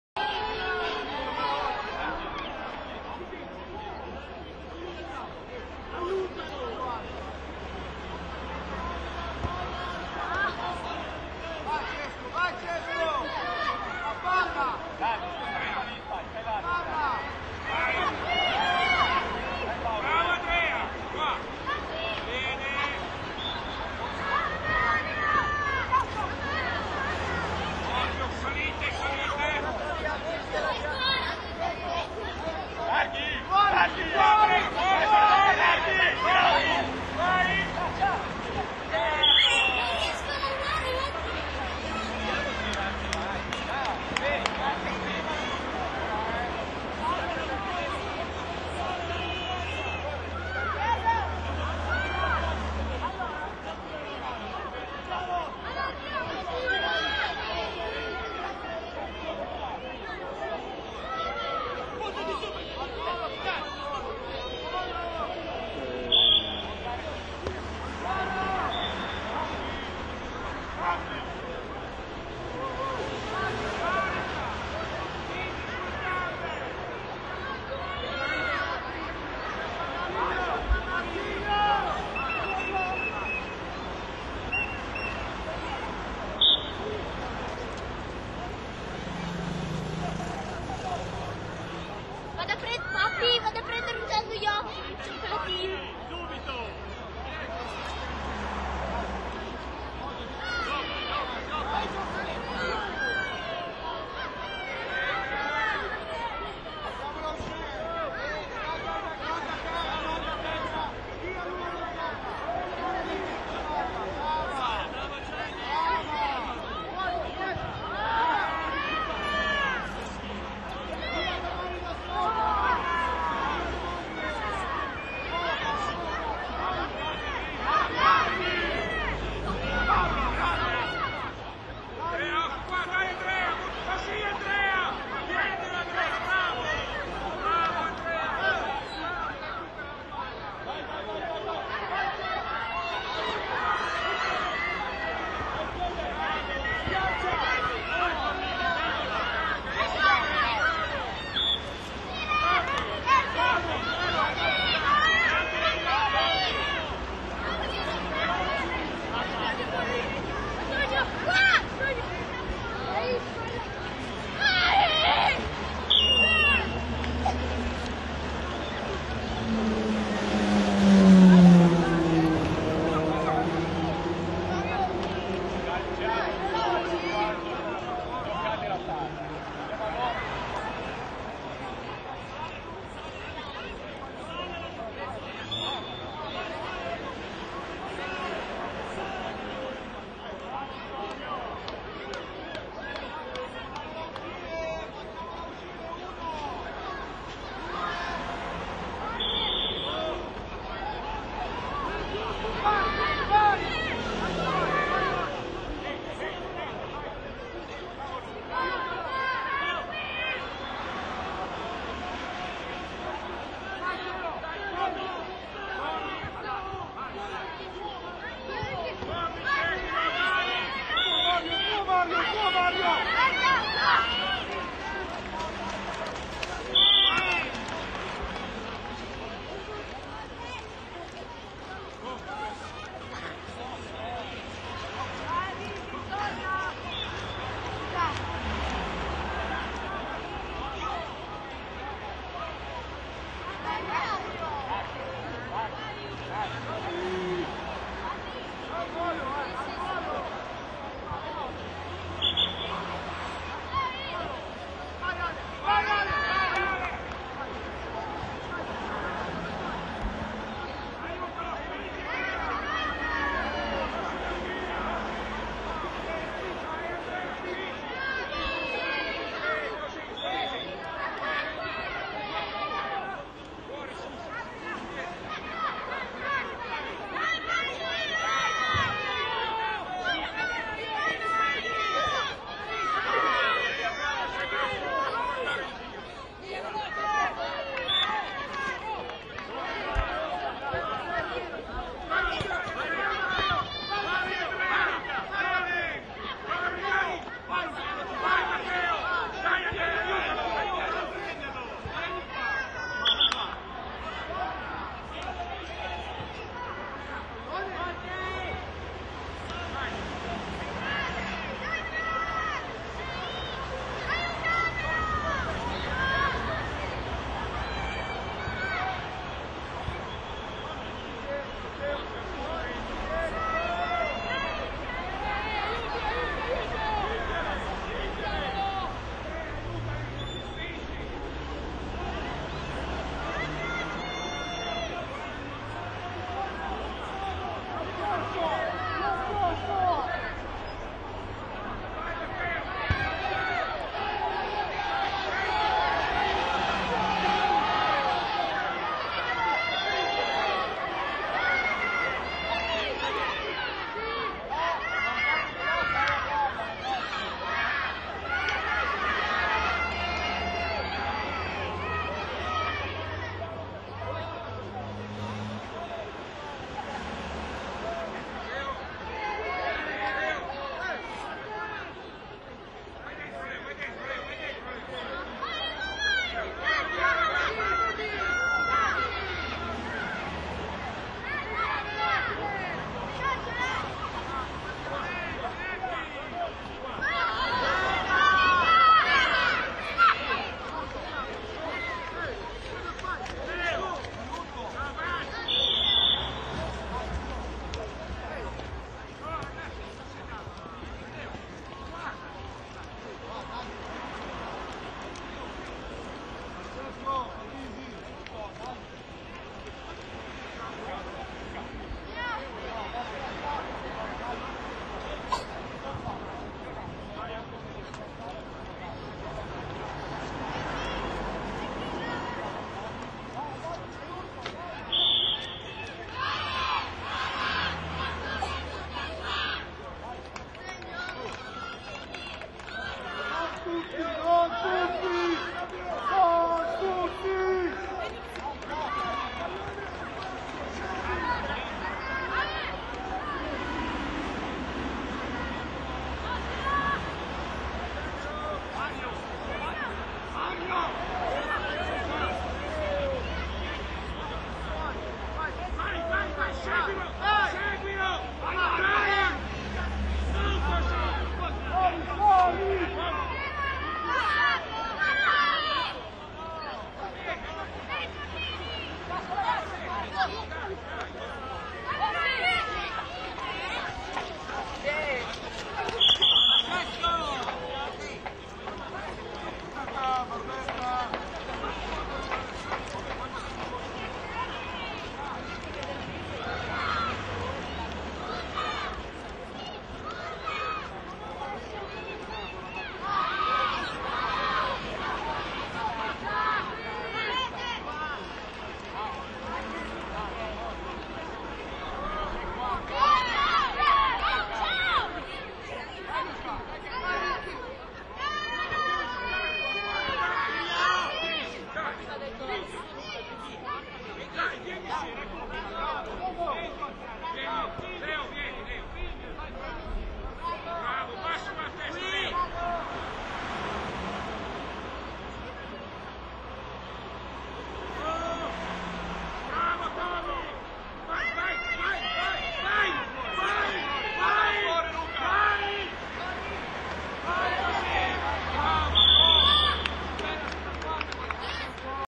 acqui-alessandria-1di2.wmv
L'Acqui Terme Rugby under 12 incontra l'Alessandria, Alessandria 21 Febbraio 2010 (prima parte)